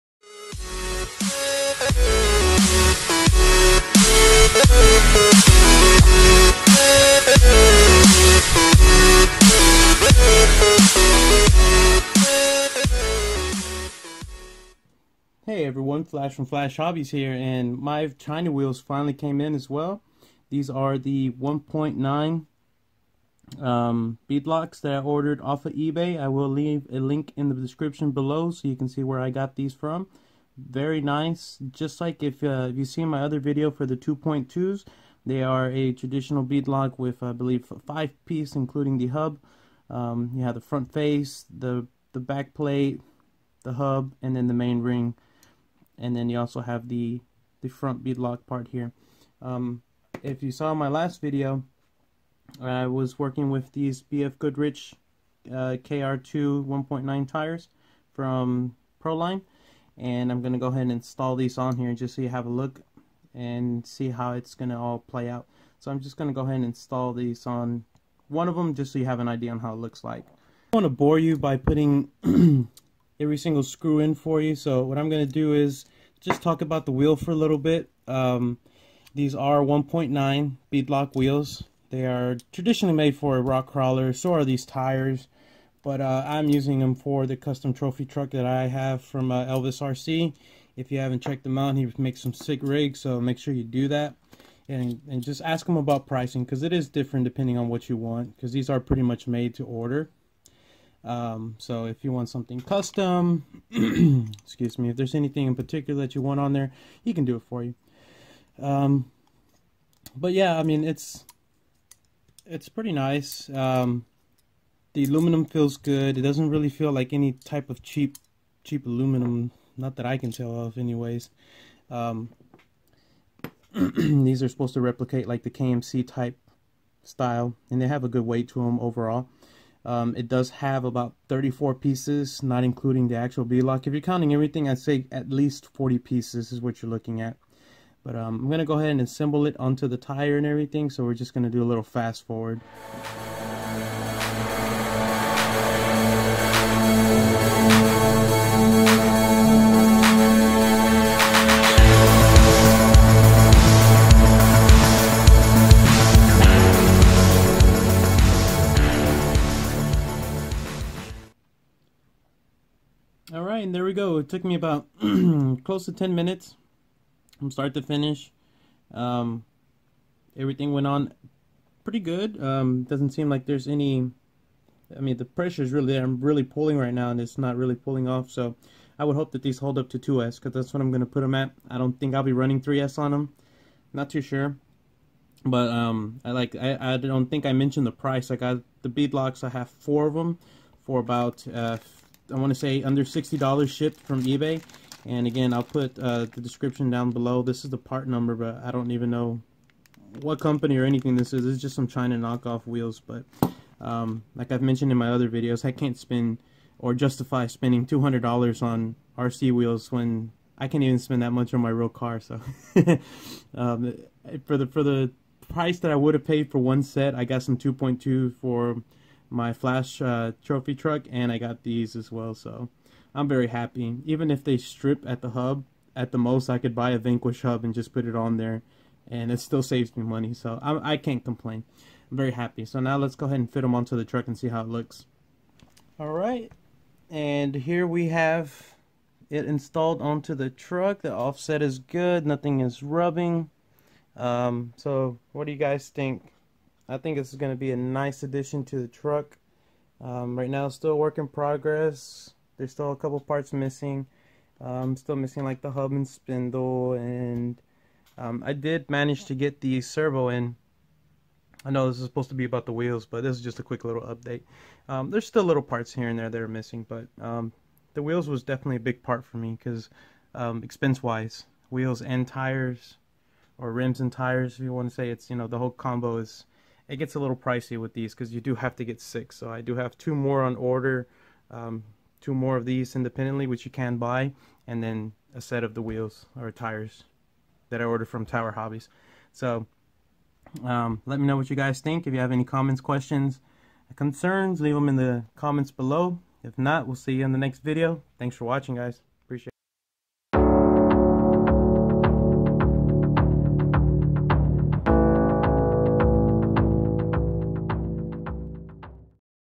EBAY 1.9 KMC Knockoff Beadlocks?
Once again I do the dirty work, so you don't have to. These are 1.9 KMC Knockoff Beadlocks.

If you are interested in these, get them